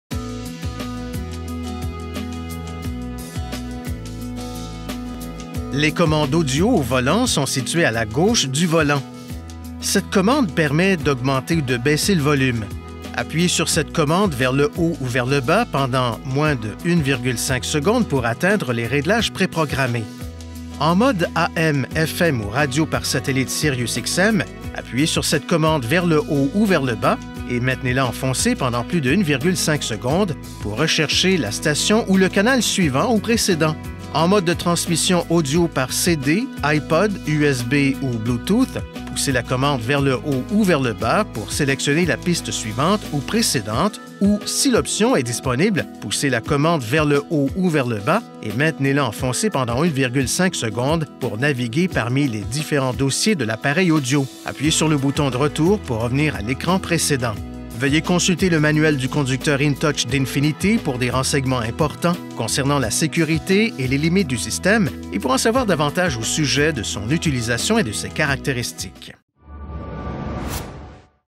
INFINITI – Commandes audio au volant
Les commandes audio au volant sont situées à la gauche du volant. Cette commande permet d’augmenter ou de baisser le volume. Appuyez sur cette commande vers le haut ou vers le bas pendant moins de 1,5 seconde pour atteindre les réglages préprogrammés. En mode AM/FM ou radio par satellite SiriusXM, appuyez sur cette commande vers le haut ou vers le bas et maintenez-la enfoncée pendant plus de 1,5 seconde pour rechercher la station ou le canal suivant ou précédent. En mode de transmission audio par CD, iPod, USB ou Bluetooth, poussez la commande vers le haut ou vers le bas pour sélectionner la piste suivante ou précédente ou, si l’option est disponible, poussez la commande vers le haut ou vers le bas et maintenez-la enfoncée pendant 1,5 seconde pour naviguer parmi les différents dossiers de l’appareil audio. Appuyez sur le bouton de retour pour revenir à l’écran précédent.

Veuillez consulter le manuel du conducteur InTouchMC d’INFINITI pour des renseignements importants concernant la sécurité et les limites du système, et pour en savoir davantage au sujet de son utilisation et de ses caractéristiques.